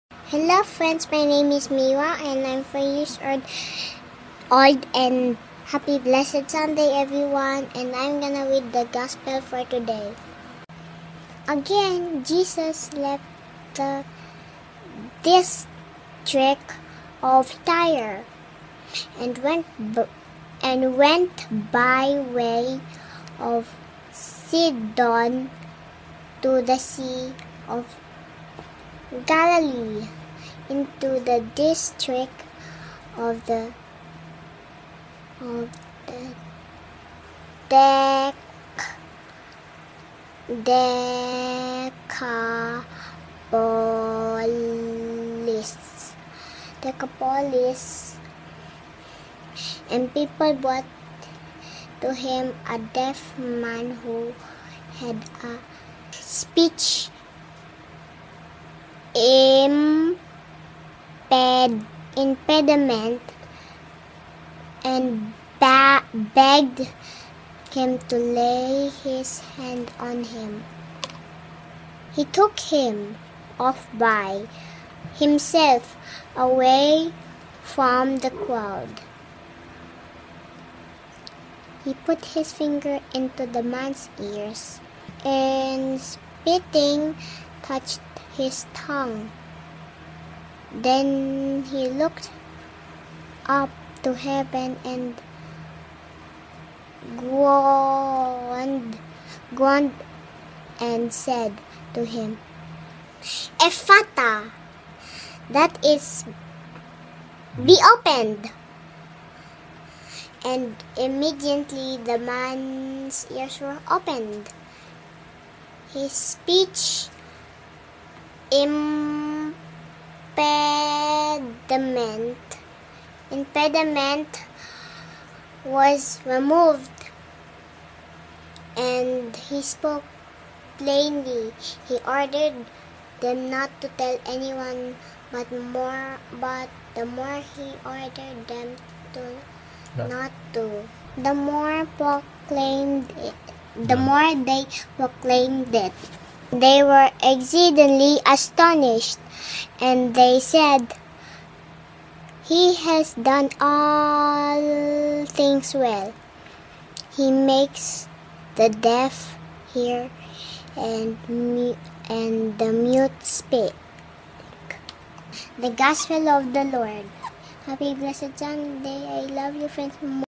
Gospel Reading Sept 05, 2021
Here is the gospel reading for today, read by a 4-year old kid. Have a blessed Sunday everyone!